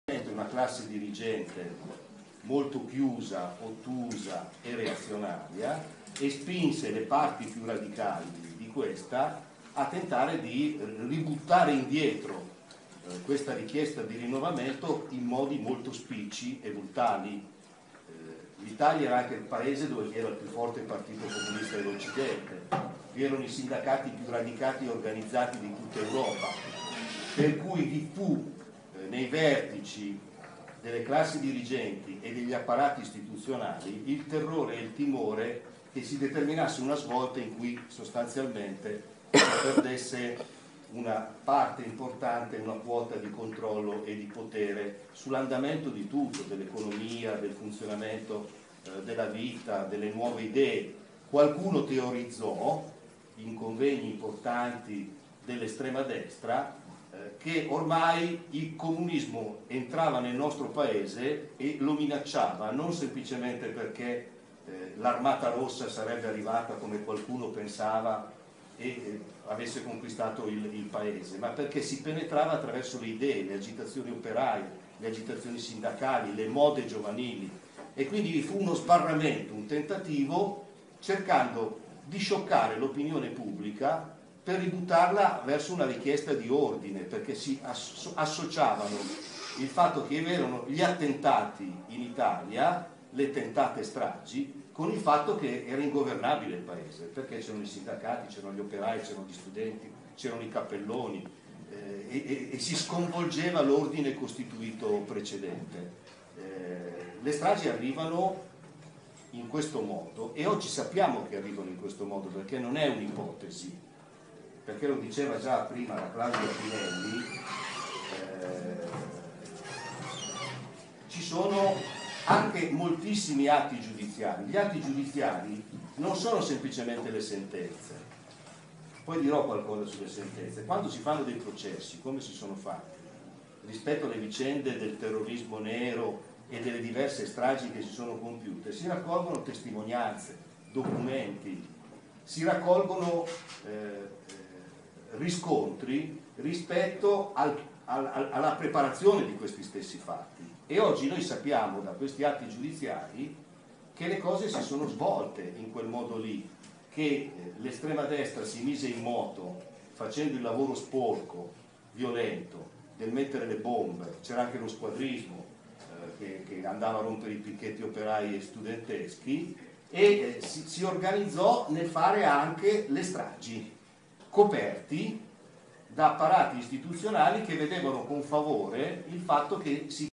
BoccioniOccupato Pt 3 La minaccia del comunismo, le stragi, gli apparati istituzionali
Collettivo su Strategia della Tensione con Saverio Ferrari e Claudia Pinelli!